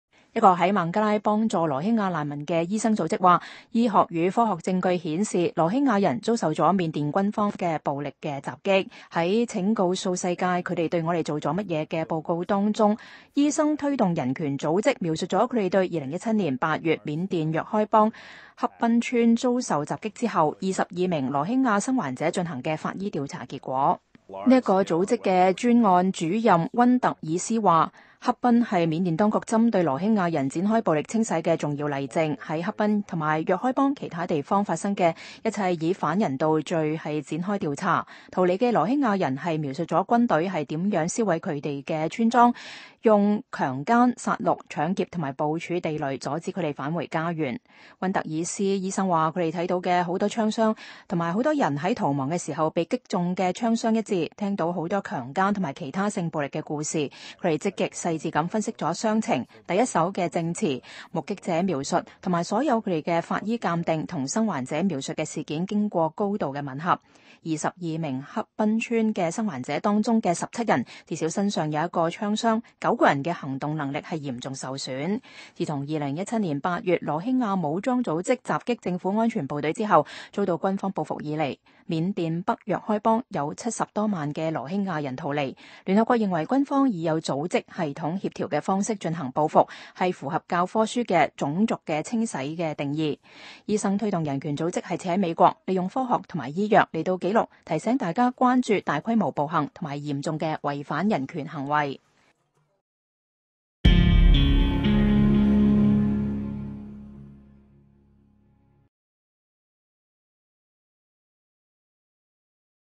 醫生組織呼籲調查緬甸是否對羅興亞人犯下人道罪
一個在孟加拉幫助羅興亞難民的醫生組織說，醫學與科學證據顯示羅興亞人遭受了緬甸軍方與非軍方的暴力襲擊。在“請告訴世界他們對我們做了什麼“的報告中，醫生推動人權組織描述了他們對2017年8月緬甸若開邦恰賓村遭受襲擊後的22名羅興亞倖存者進行的法醫調查結果。